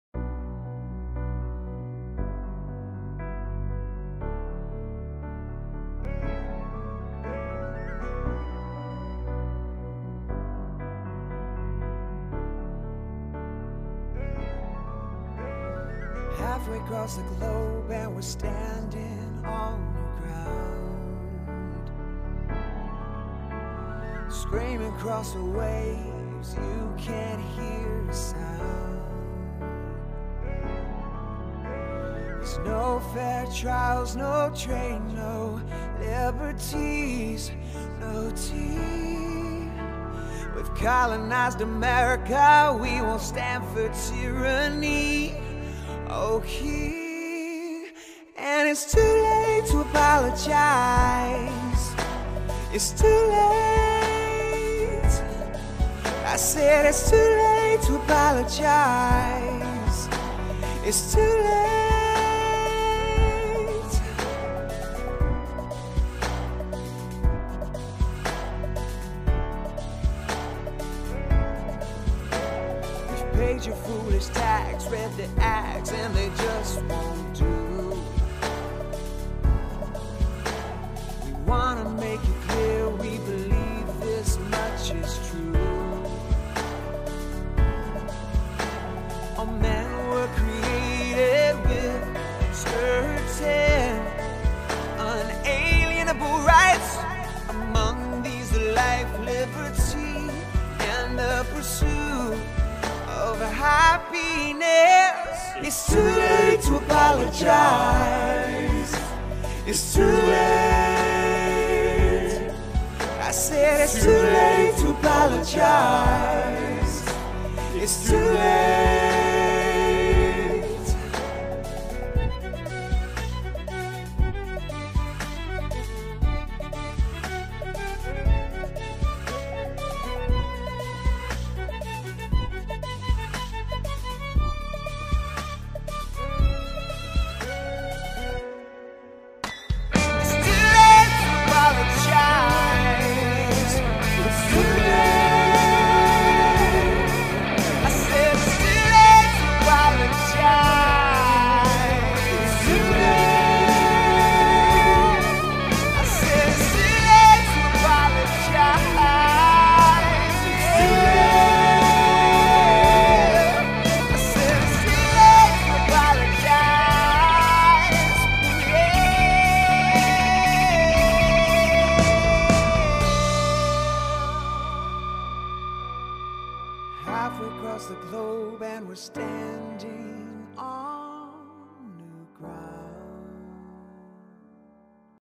A Declaration;; John Adams
Anyway, this is really bad, but the idea occurred to be LITERALLY TODAY (. yesterday? On the Fourth, at any rate) so I tried to throw it together in time to be uploaded on Independence Day itself, and failed. Its rushed and not great but I'm more or less satisfied.